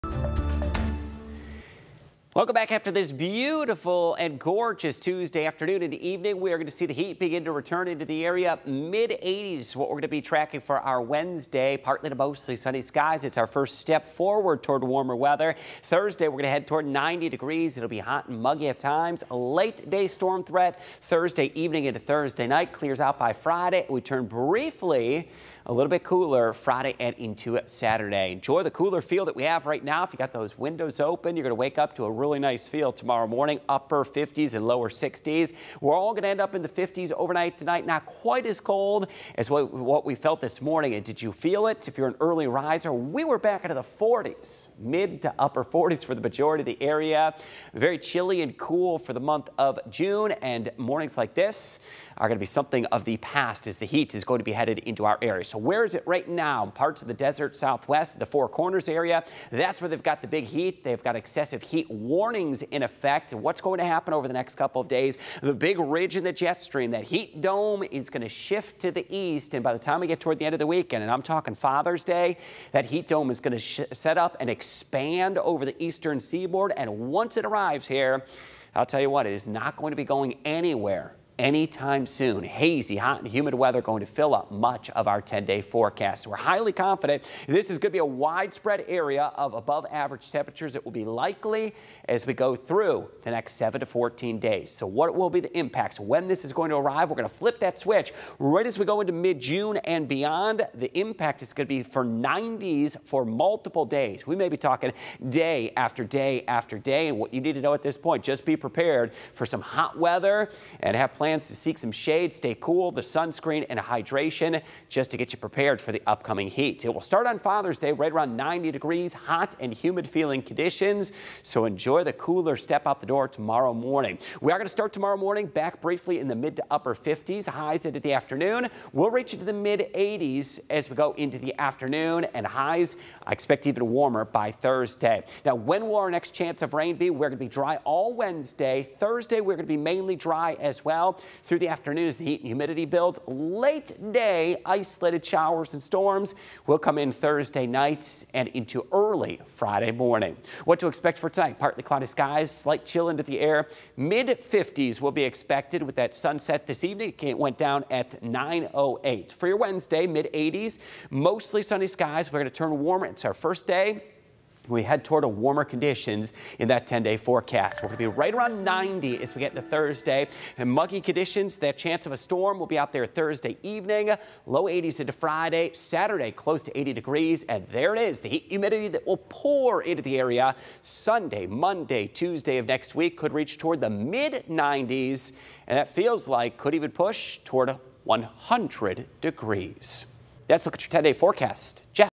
Wednesday morning clear in 50s before warm, sunny day | WTOL 11 Weather
Mostly sunny skies and warmer weather will be expected on Wednesday with temperatures in the mid-80s.